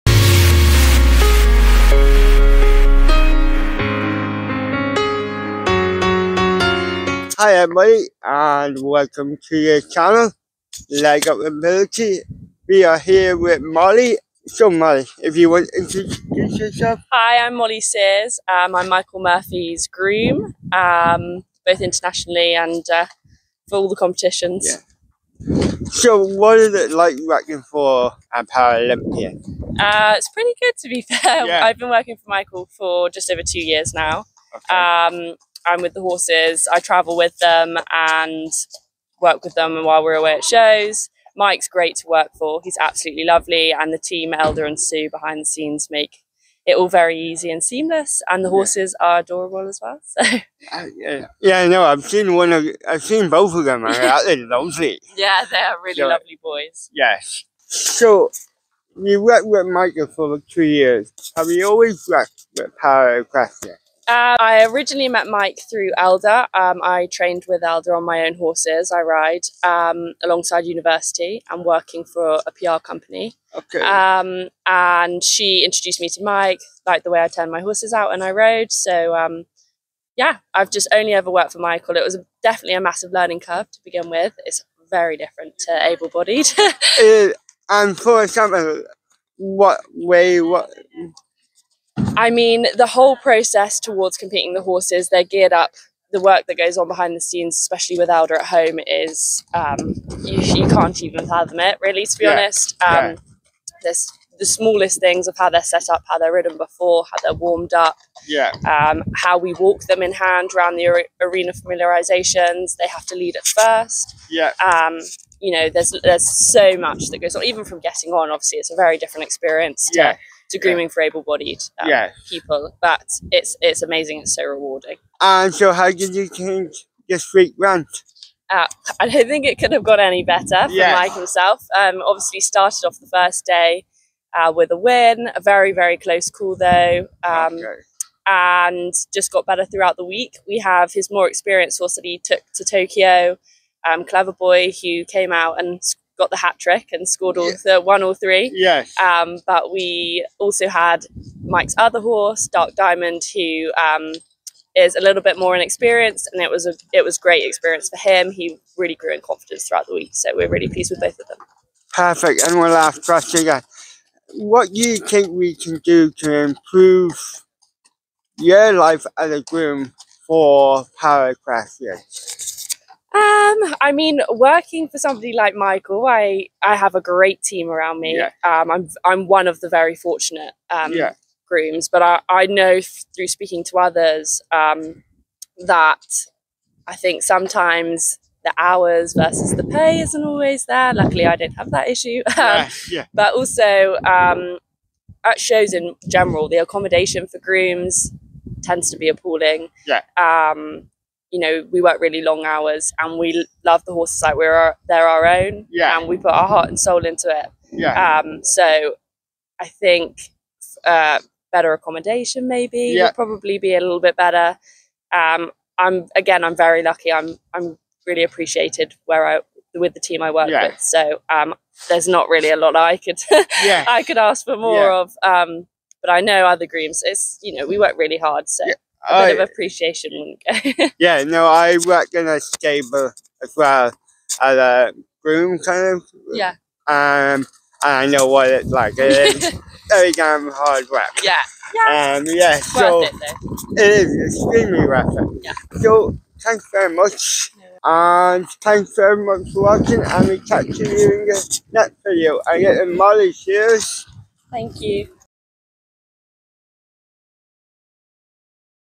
International Groom Mollie Sears
Hi everybody,

Welcome to the LegUpAbility channel and the final in the Addington series of interviews. The LegUpAbility Development Team had an exciting week in Addington at our first-ever FEI International ParaDressage Competition.

In this final video, we talk to International Groom Mollie Sears, who has worked with Paralympian Michael Murphy for the last three years.   Mollie discusses what it's like working for a Paralympian and highlights the hard work behind-the-scenes grooms do to contribute to the success of Paraequestrians.